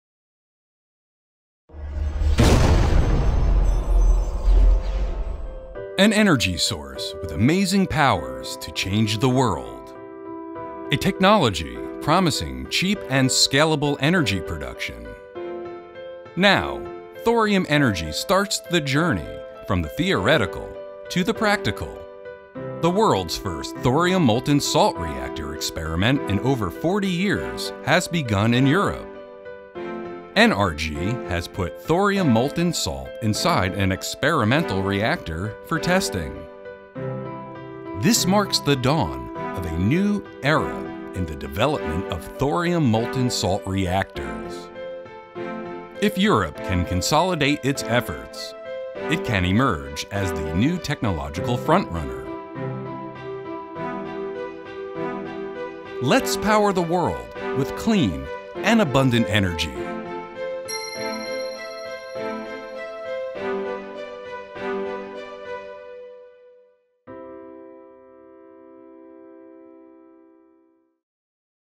World’s First Thorium Molten Salt Experiment in over 45 Years
Europe have begun the world’s first thorium molten salt reactor experiment in over 45 years. With this, Europe has entered the Thorium Energy race!
Who will be next?

Thorium is a natural element that can power the world for thousands of years with clean energy! You can hold your entire life’s energy supply in the palm of your hand with thorium.
It has been estimated that 30 times more thorium than what is required to power the entire world’s energy demand is mined as a by-product and thrown away every year.
The race to commercialise thorium is on with China in the lead followed by India, Indonesia and now Europe.
At ThoriumEnergyWorld.com we strive to power the world with Thorium Energy!

Credits to Jay Stern for amazing video skills!

Narrated by Jay Stern